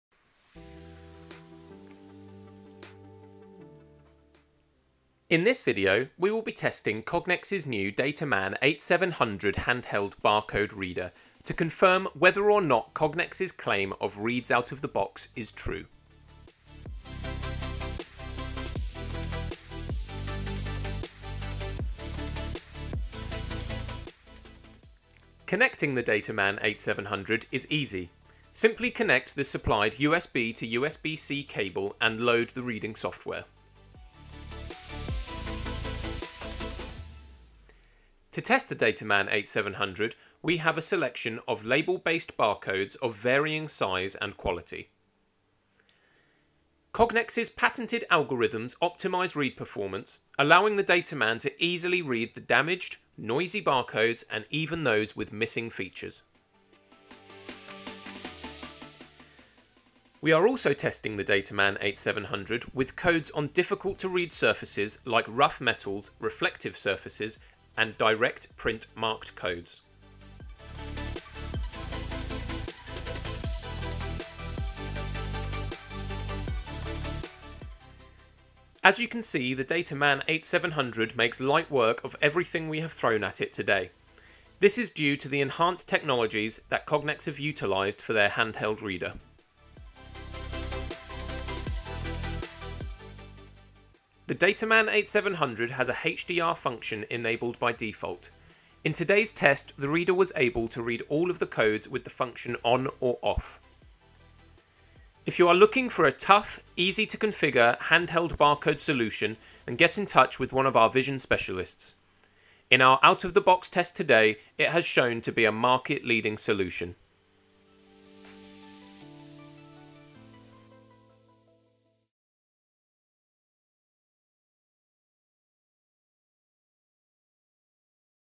Cognex Dataman 8700 Rugged Handheld Barcode Scanner Demonstration
In this video, we are testing the Dataman 8700 from Cognex. Kam our vision specialist runs a series of tests to see how the Dataman 8700 performs straight out of the box.